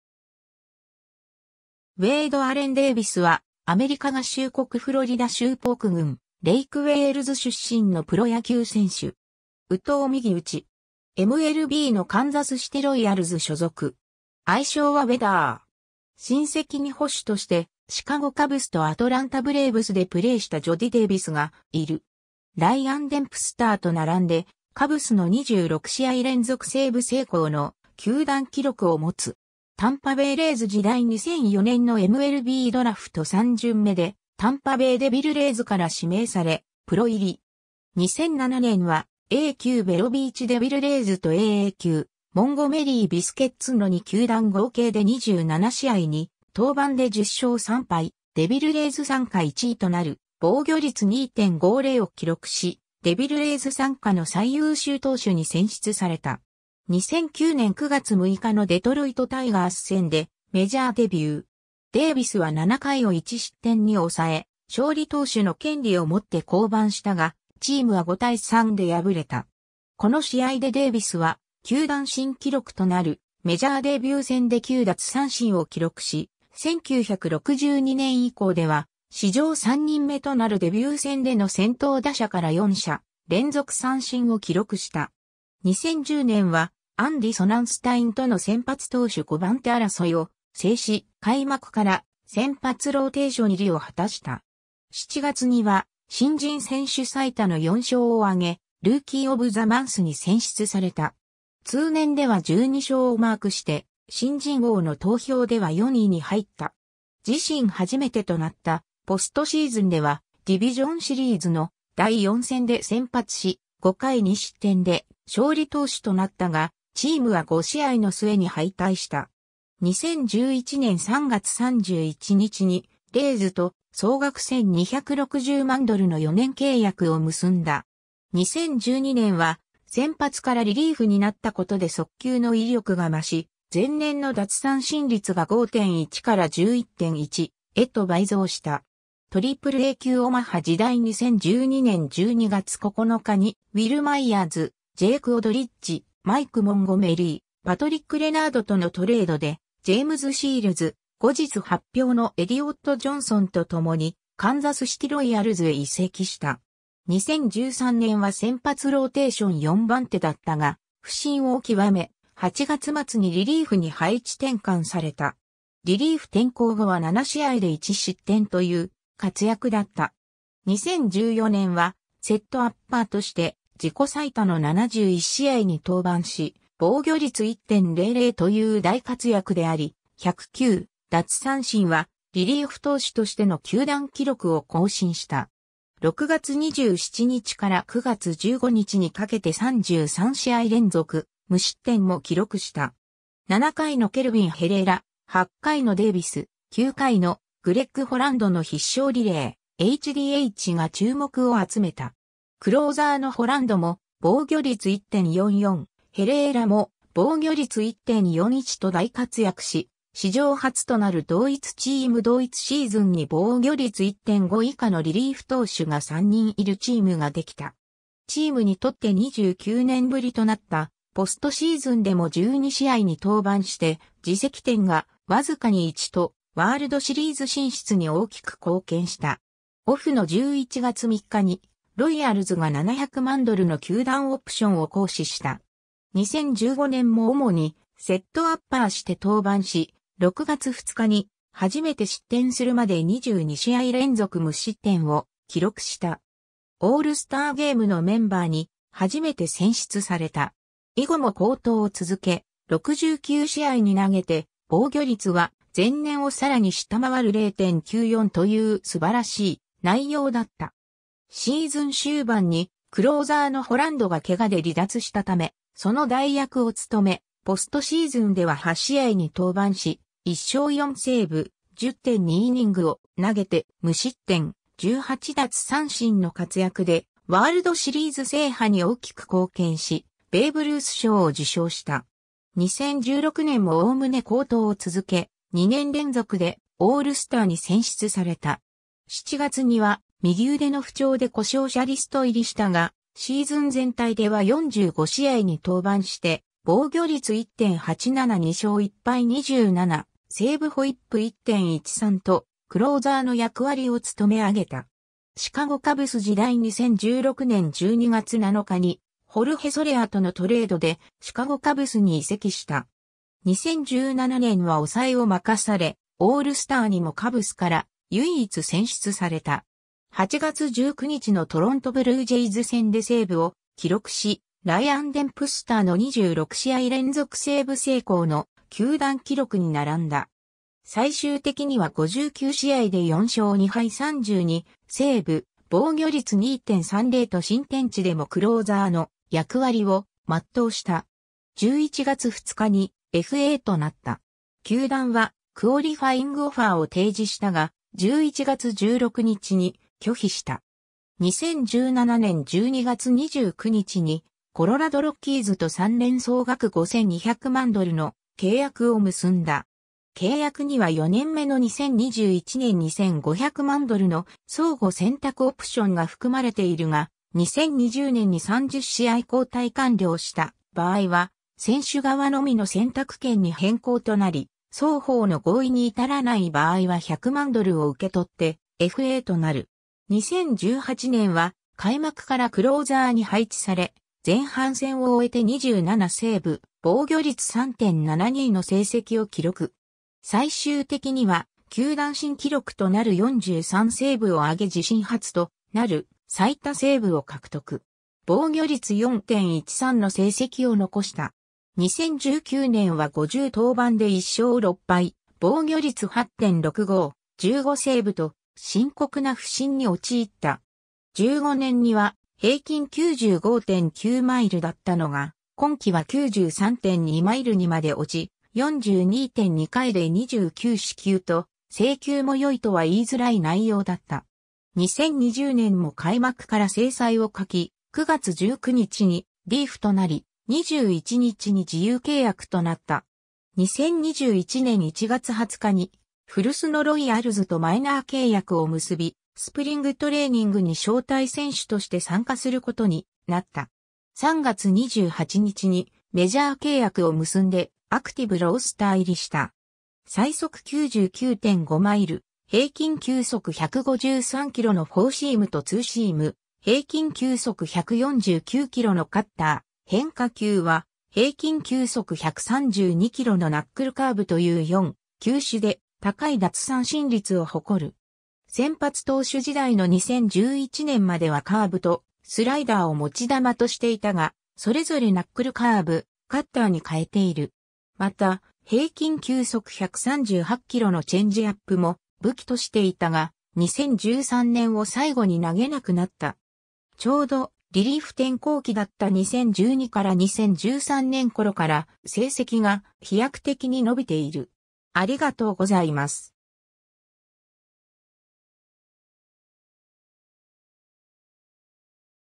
ウェイド・デービス (野球)
カンザスシティ・ロイヤルズ傘下の選手
ウェイド・アレン・デービス（Wade Allen Davis, 1985年9月7日 - ）は、アメリカ合衆国フロリダ州ポーク郡レイクウェールズ出身のプロ野球選手（投手）。
右投右打。
MLBのカンザスシティ・ロイヤルズ所属。
愛称はウェダー。
 親戚に捕手としてシカゴ・カブスとアトランタ・ブレーブスでプレーしたジョディ・デービスがいる。
 ライアン・デンプスターと並んで、カブスの26試合連続セーブ成功の球団記録を持つ。
 タンパベイ・レイズ時代 （2012年9月13日） 2004年のMLBドラフト3巡目（全体75位）でタンパベイ・デビルレイズから指名され、プロ入り。
 2007年はA+級ベロビーチ・デビルレイズとAA級モンゴメリー・ビスケッツの2球団合計で27試合に登板で10勝3敗、デビルレイズ傘下1位となる防御率2.50を記録し、デビルレイズ傘下の最優秀投手に選出された。
 2009年9月6日のデトロイト・タイガース戦でメジャーデビュー。
デービスは7回を1失点に抑え、勝利投手の権利を持って降板したが、チームは5対3で敗れた。
この試合でデービスは球団新記録となるメジャーデビュー戦で9奪三振を記録し、1962年以降では史上3人目となるデビュー戦での先頭打者から4者連続三振を記録した。
 2010年はアンディ・ソナンスタインとの先発投手5番手争いを制し、開幕から先発ローテーション入りを果たした。
7月には新人選手最多の4勝を挙げ、ルーキー・オブ・ザ・マンスに選出された。
通年では12勝をマークして、新人王の投票では4位に入った。
自身初めてとなったポストシーズンでは、ディビジョンシリーズの第4戦で先発し、5回2失点で勝利投手となったが、チームは5試合の末に敗退した。
 2011年3月31日にレイズと総額1260万ドルの4年契約（2015年・700万ドル、2016年・800万ドル、2017年・1000万ドルの球団オプション付き）を結んだ。
 2012年は、先発からリリーフになったことで速球の威力が増し、前年の奪三振率が5.1から11.1へと倍増した。
 AAA級オマハ時代 （2016年8月30日） 2012年12月9日にウィル・マイヤーズ、ジェイク・オドリッジ、マイク・モンゴメリー、パトリック・レナードとのトレードで、ジェームズ・シールズ、後日発表のエリオット・ジョンソンとともにカンザスシティ・ロイヤルズへ移籍した。
 2013年は先発ローテーション4番手だったが不振を極め、8月末にリリーフに配置転換された。
リリーフ転向後は7試合（10イニング）で1失点という活躍だった。
 2014年はセットアッパーとして自己最多の71試合に登板し、防御率1.00という大活躍であり、109奪三振はリリーフ投手としての球団記録を更新した。
6月27日から9月15日にかけて33試合連続無失点も記録した。
7回のケルビン・ヘレーラ、8回のデービス、9回のグレッグ・ホランドの必勝リレー「HDH」が注目を集めた。
クローザーのホランドも防御率1.44、ヘレーラも防御率1.41と大活躍し、史上初となる同一チーム同一シーズンに防御率1.5以下のリリーフ投手（60イニング以上）が3人いるチームができた。
チームにとって29年ぶりとなったポストシーズンでも12試合（14.1イニング）に登板して、自責点がわずかに1と、ワールドシリーズ進出に大きく貢献した。
オフの11月3日にロイヤルズが700万ドルの球団オプションを行使した。
 2015年も主にセットアッパーして登板し、6月2日に初めて失点するまで22試合連続無失点を記録した。
オールスターゲームのメンバーに初めて選出された。
以後も好投を続け、69試合に投げて防御率は前年を更に下回る0.94という素晴らしい内容だった。
シーズン終盤にクローザーのホランドが怪我で離脱したため、その代役を務め、ポストシーズンでは8試合に登板し、1勝4セーブ、10.2イニングを投げて、無失点、18奪三振の活躍でワールドシリーズ制覇に大きく貢献し、ベーブ・ルース賞を受賞した。
 2016年も概ね好投を続け、2年連続でオールスターに選出された。
7月には右腕の不調...